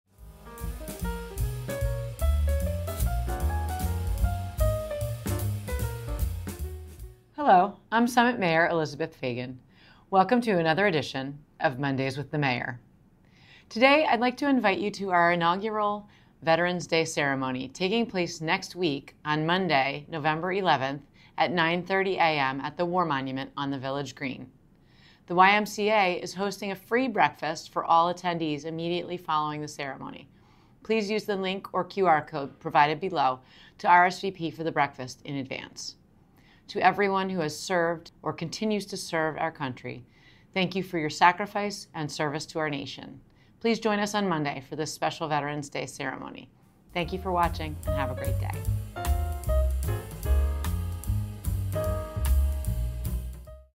Mondays with the Mayor | November 4, 2024
Mayor Elizabeth Fagan, MD, shares information on city news, initiatives, events, and other timely topics every Monday.

RSVP for the free breakfast at the YMCA following the Veterans Day ceremony